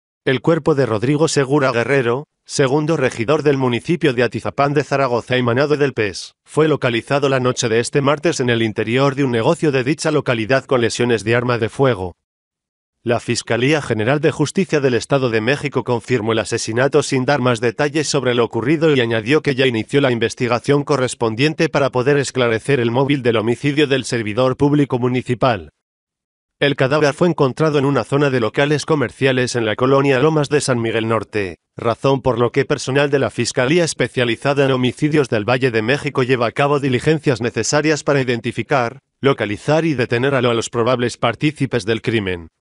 Matan a regidor de Atizapán de Zaragoza
El cuerpo de Rodrigo Segura Guerrero, segundo regidor del municipio de Atizapán de Zaragoza emanado del PES, fue localizado la noche de este martes en el interior de un negocio de dicha localidad con lesiones de arma de fuego.

La Fiscalía General de Justicia del Estado de México confirmó el asesinato sin dar más detalles sobre lo ocurrido y añadió que ya inició la investigación correspondiente para poder esclarecer el móvil del homicidio del servidor público municipal.

El cadáver fue encontrado en una zona de locales comerciales en la colonia Lomas de San Miguel Norte, razón por lo que personal de la Fiscalía Especializada en Homicidios del Valle de México lleva a cabo diligencias necesarias para identificar, localizar y detener al o a los probables partícipes del crimen.